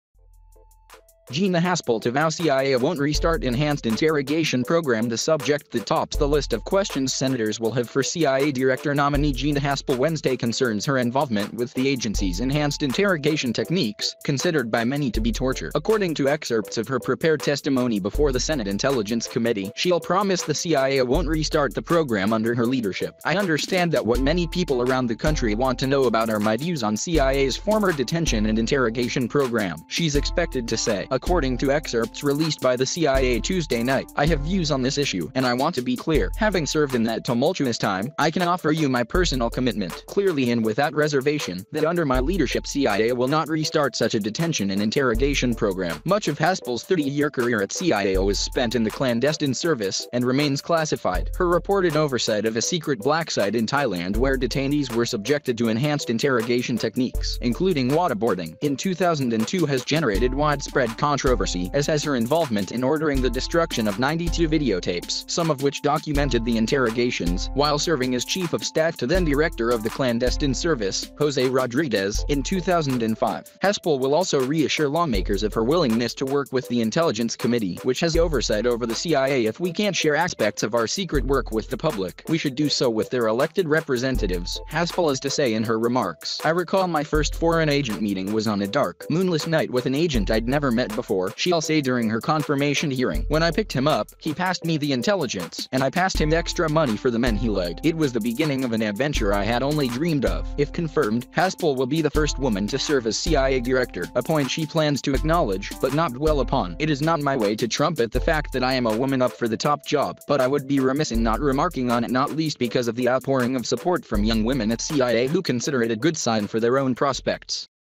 Gina Haspel to vow CIA won't restart enhanced interrogation program
The subject that tops the list of questions senators will have for CIA Director nominee Gina Haspel Wednesday concerns her involvement with the agency's "enhanced interrogation techniques," considered by many to be torture. According to excerpts of her prepared testimony before the Senate Intelligence Committee, she'll promise the CIA won't restart the program under her leadership.
"I understand that what many people around the country want to know about are my views on CIA's former detention and interrogation program," she's expected to say, according to excerpts released by the CIA Tuesday night. "I have views on this issue, and I want to be clear. Having served in that tumultuous time, I can offer you my personal commitment, clearly and without reservation, that under my leadership CIA will not restart such a detention and interrogation program."
Much of Haspel's 30-year career at CIA was spent in the clandestine service, and remains classified. Her reported oversight of a secret "black site" in Thailand where detainees were subjected to enhanced interrogation techniques, including waterboarding, in 2002 has generated widespread controversy, as has her involvement in ordering the destruction of 92 videotapes – some of which documented the interrogations – while serving as chief of staff to then-director of the clandestine service, Jose Rodriguez, in 2005.